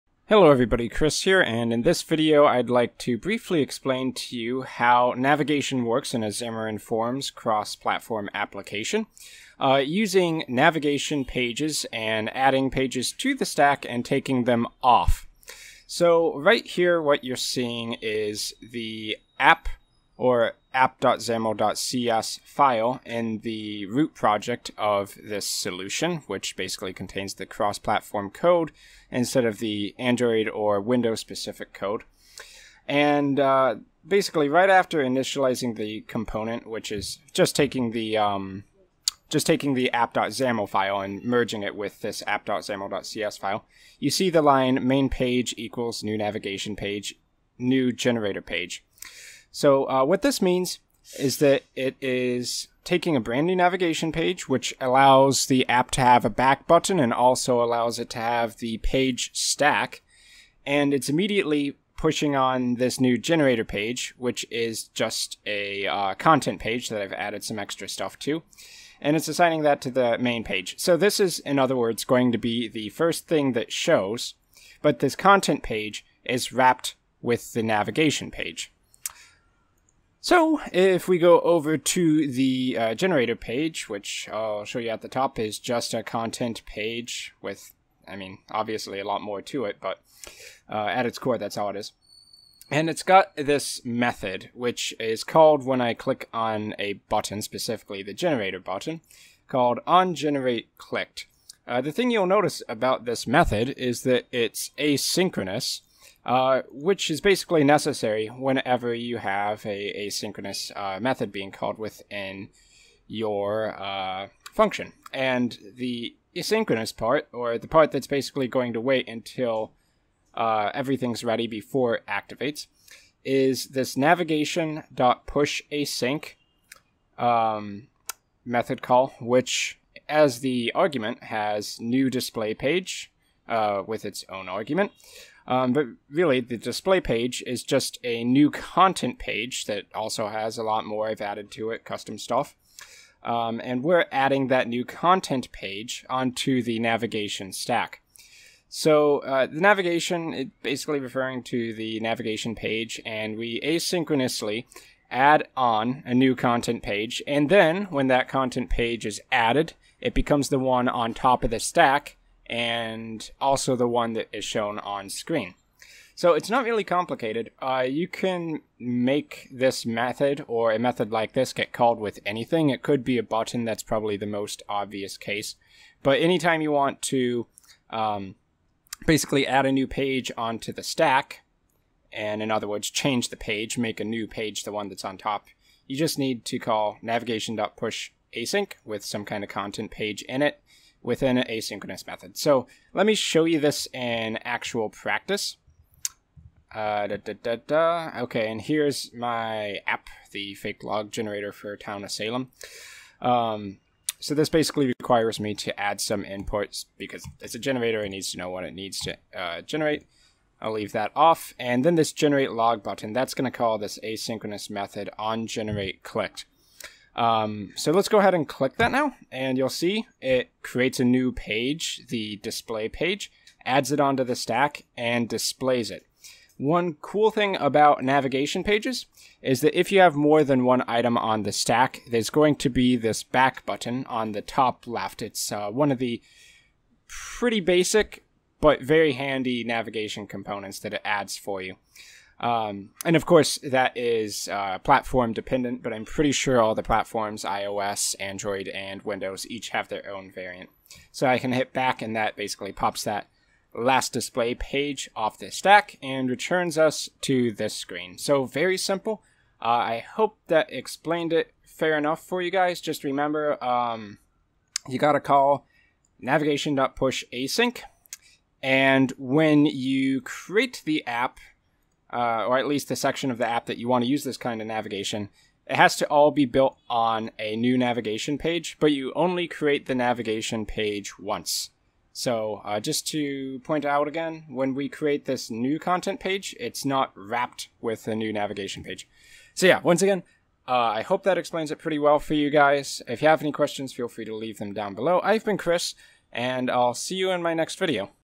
Basic Page Navigation - Navigation Page and PushAsync | Xamarin Forms C# Android iOS Tutorial
Learn how to allow your app to navigate between content pages by setting up a navigation page and calling the proper functions.

Learn Video Editing - Complete Guide to DaVinci Resolve Course ($5 Off)